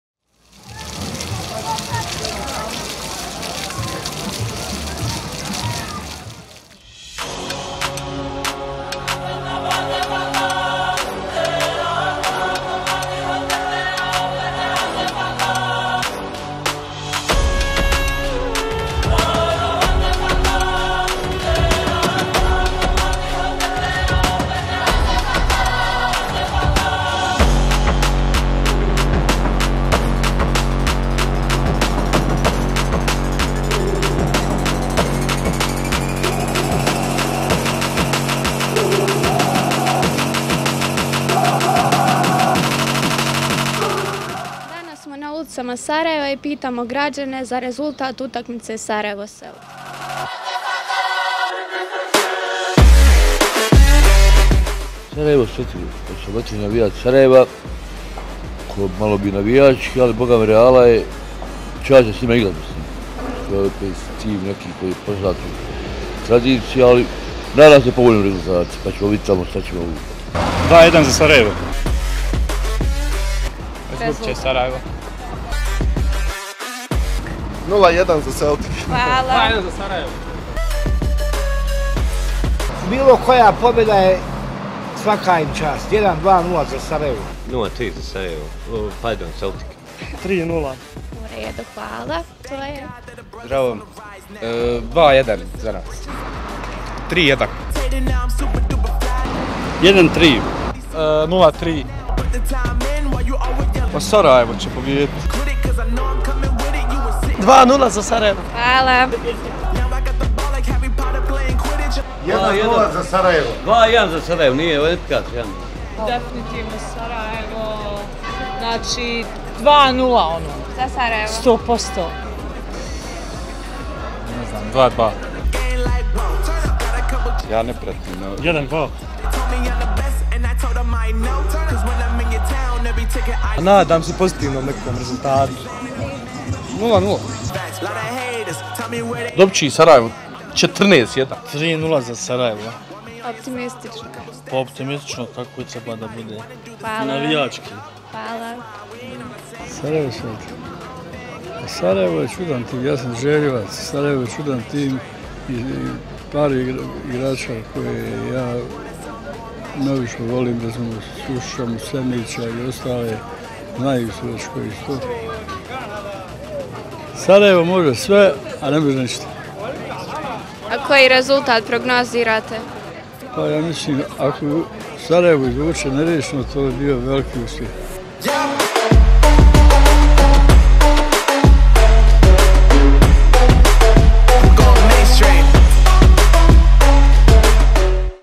Historijski susret: Sarajlije prognoziraju rezultat utakmice Sarajevo - Celtic
Historijska utakmica odigrava se sutra u našem glavnom gradu. Osvajač duple krune u našoj zemlji za prošlu sezonu, FK Sarajevo, dočekuje velikana svjetskog fudbala i osvajača škotskog Premiershipa, FK Celtic.

Pitali smo Sarajlije šta misle kako će susret da završi, u korist koga. Čuli smo neke zaista zanimljive odgovore, a u videu koji vam donosimo možete i vi da ih čujete.

Zahvaljujemo se našim sagovornicima na izdvojenom vremenu.

Posebna zahvala pripada našim prijateljima koji su podržali projekat, koji je na taj način oživio, a to su: Pekare "Irnes" d.o.o.

Novinar: Delina Voloder
Kamera/ montaža: Akif Suljić